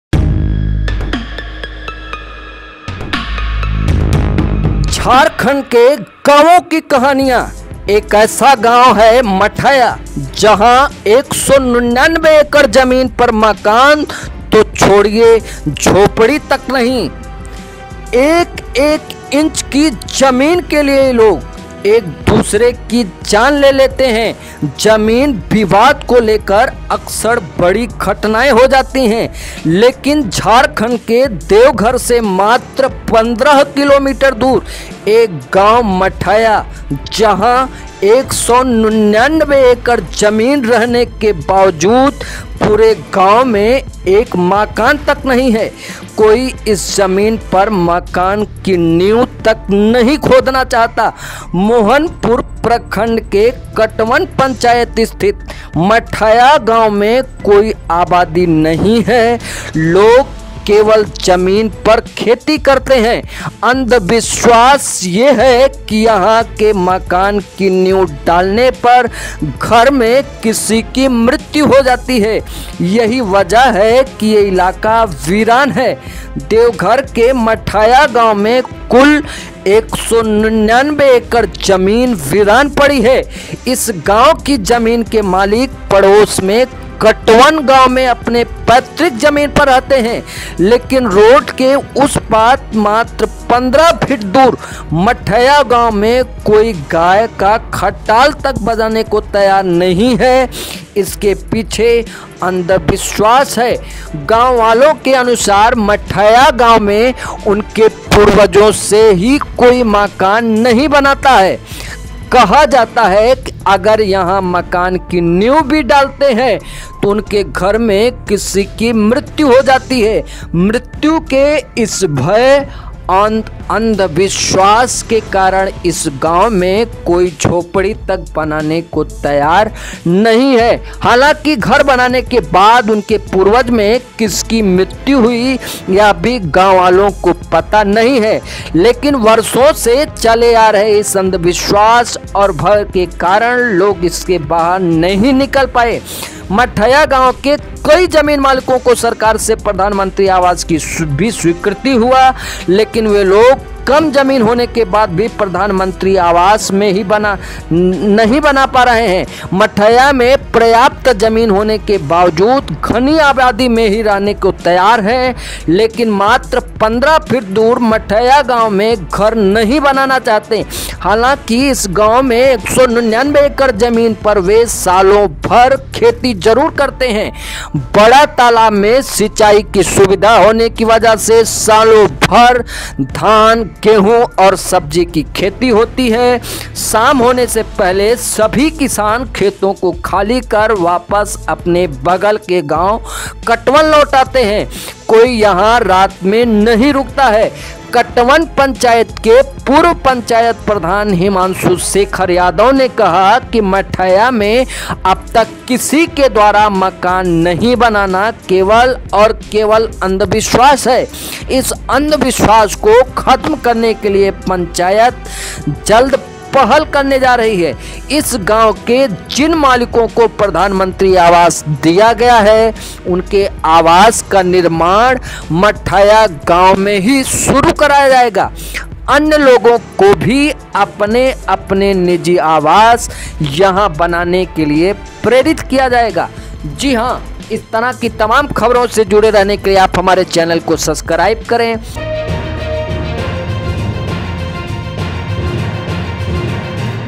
झारखंड के गांवों की कहानियां: एक ऐसा गांव मठेया, जहां 199 एकड़ जमीन पर मकान तो छोड़िए झोपड़ी तक नहीं
झारखंड के गांवों की कहानियां : एक ऐसा गांव है मठेया, जहां 199 एकड़ जमीन पर मकान तो छोड़िए झोपड़ी तक नहीं

People kill each other for every inch of land. There are often big incidents regarding land dispute, but there is a village just 15 km away from Deoghar in Jharkhand, where despite having 199 acres of land, there is not even a house in the entire village. No one wants to dig even the foundation of the house on this land. There is no population in Matheya village located in Katwan Panchayat of Mohanpur block. People only cultivate the land. The superstition is that after laying the foundation of the house here, someone dies in the house. This is the reason why this area is deserted.

एक-एक इंच की जमीन के लिए लोग एक दूसरे की जान ले लेते हैं. जमीन विवाद को लेकर अक्सर बड़ी घटनाएं हो जाती हैं, लेकिन झारखंड के देवघर से मात्र 15 किलोमीटर दूर एक गांव है मठेया, जहां 199 एकड़ जमीन रहने के बावजूद पूरे गांव में एक मकान तक नहीं है. कोई इस जमीन पर मकान की नींव तक नहीं खोदना चाहता. मोहनपुर प्रखंड की कटवन पंचायत स्थित मठेया गांव में कोई आबादी नहीं है. लोग केवल जमीन पर खेती करते हैं. अंधविश्वास ये है कि यहां मकान की नींव डालने पर घर में किसी की मृत्यु हो जाती है. यही वजह है कि ये इलाका वीरान है.